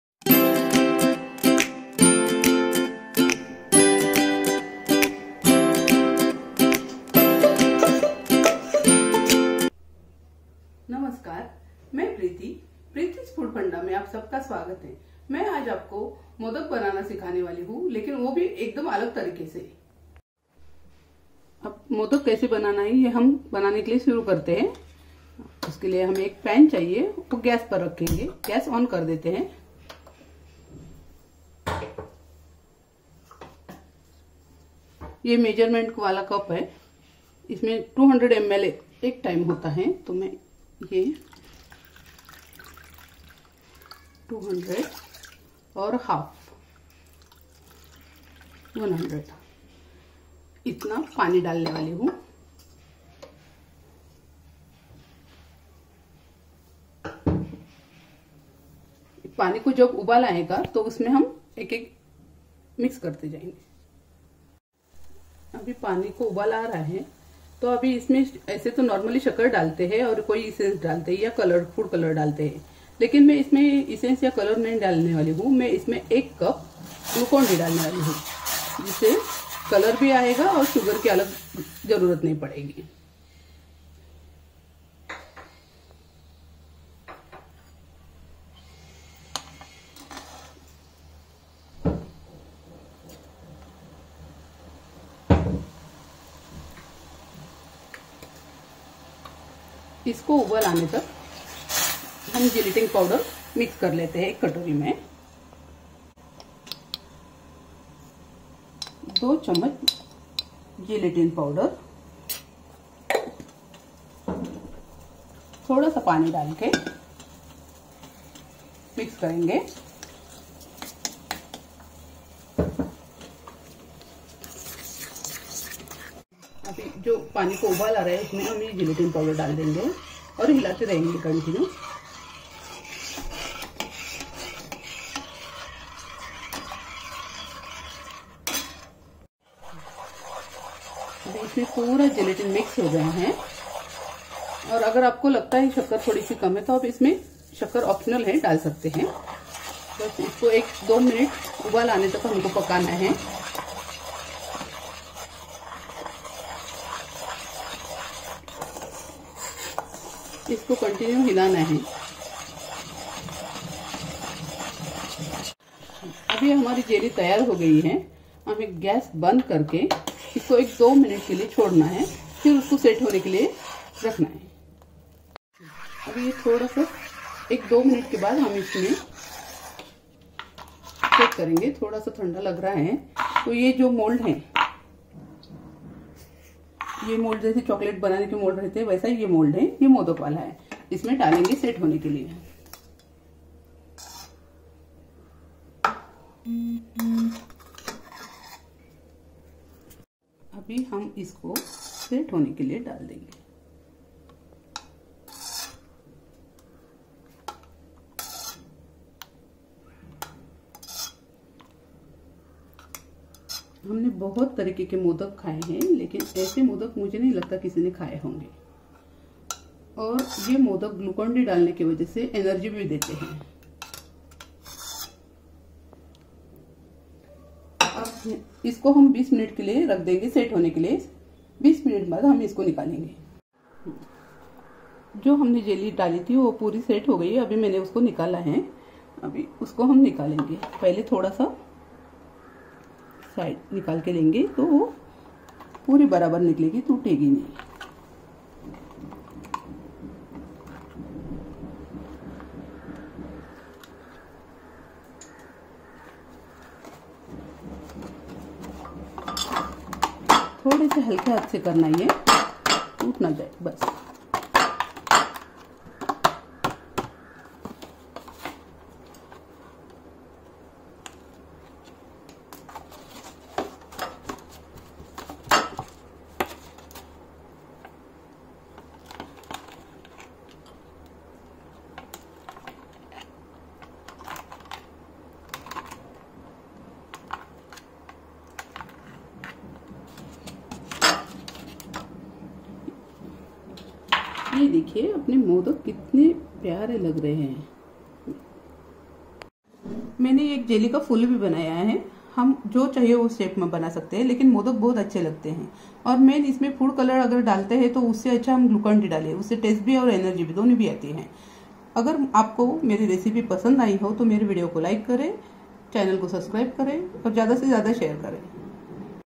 Jelly Modak
Healthy, Tasty, full of energy and unique receipe of making Jelly Modaks.

Easy to make and especially loved by kids.

Ingredients:
Veg Gelatine Powder(Butterfly company)
Glucon-D
Water